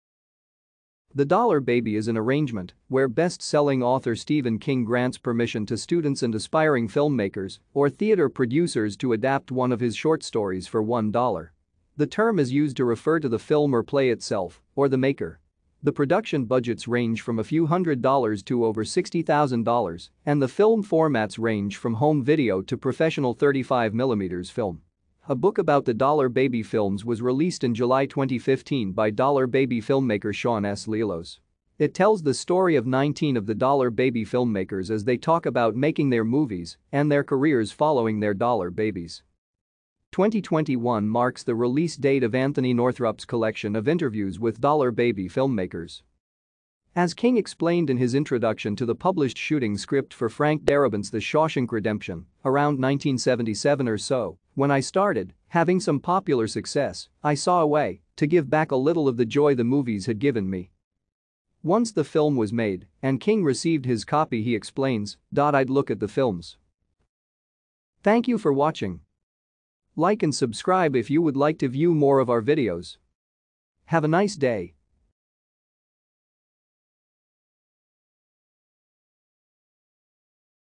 Dollar Baby
The Dollar Baby  is an arrangement where best-selling author Stephen King grants permission to students and aspiring makers or theatre producers to adapt one of his short stories for $1. The term is used to refer to the  or play itself, or the maker.  The production budgets range from a few hundred dollars to over $60,000  and the  formats range from home video to professional 35 mm . A book about the Dollar Baby s was released in July 2015 by Dollar Baby maker Shawn S. Lealos. It tells the story of 19 of the Dollar Baby makers as they talk about making their s and their careers following their Dollar Babies.